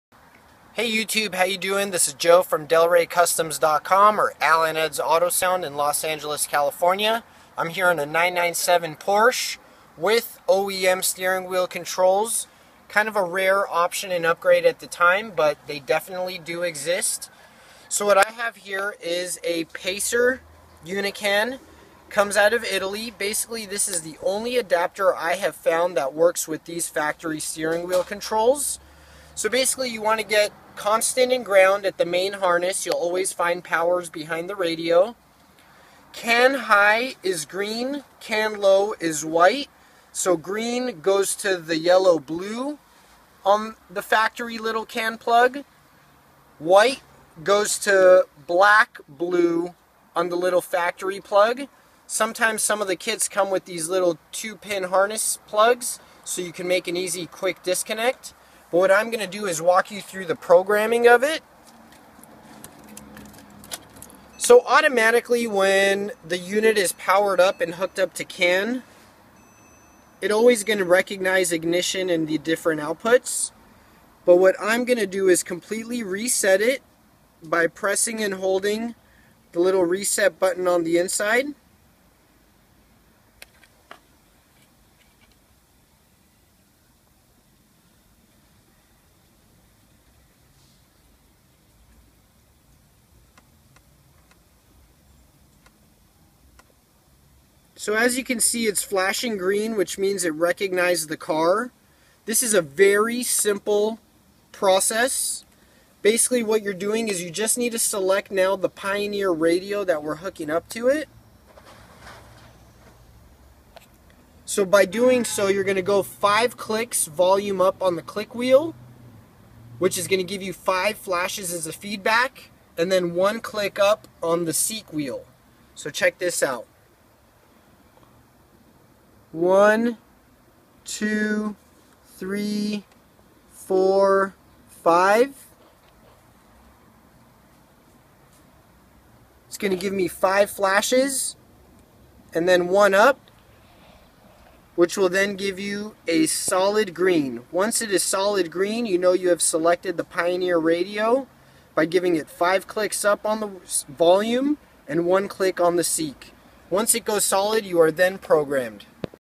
Paser unican Porsche programming steering wheel Pioneer controls
Porsche 997 carrera programming for Paser interface to retain steering wheel stereo functions.

UNICANPLUS is a steering wheel commands interface able to control aftermarket radios by managing both Canbus, K-bus protocols.
UNICANPLUS has car Canbus protocols and radio protocols built in its memory.
To select the right car protocol, you only have to connect the unit to Canbus car net; radio protocol must be selected from the steering wheel commands.
UNICANPLUS is self-programming!
The unit is upgradeable!
UNICANPLUS has same technical specifications like UNICAN.